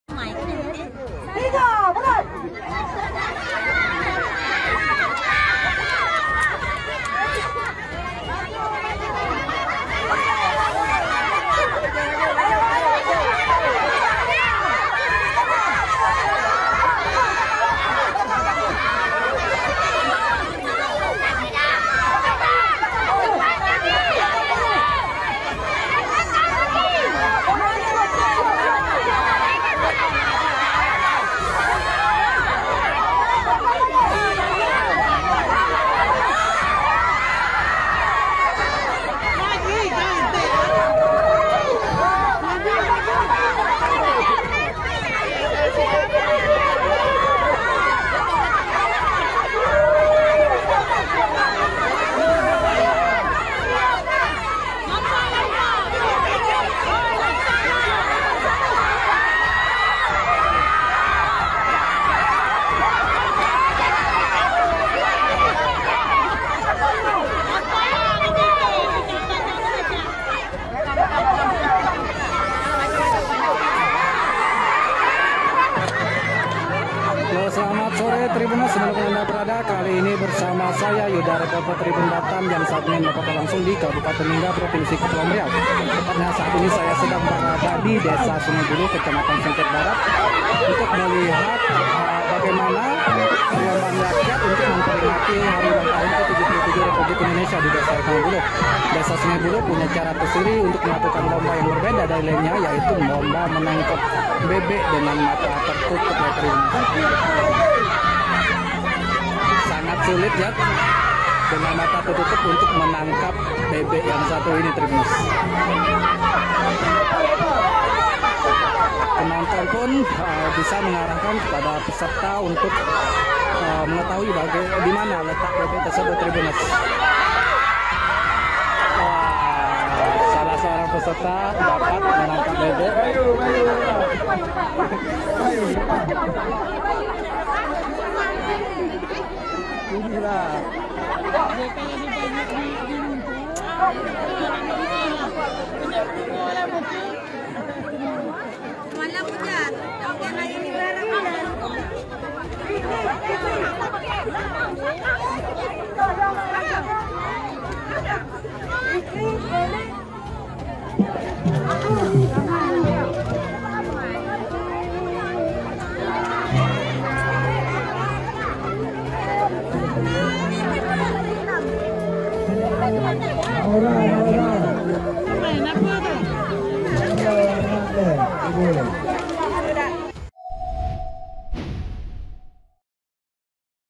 Keseruan Lomba Tangkap Bebek di HUT Kemerdekaan RI di Lingga
Reporter: Yuda

Keseruan Lomba Tangkap Bebek pada HUT RI di Desa Sungai Buluh, Lingga

editing W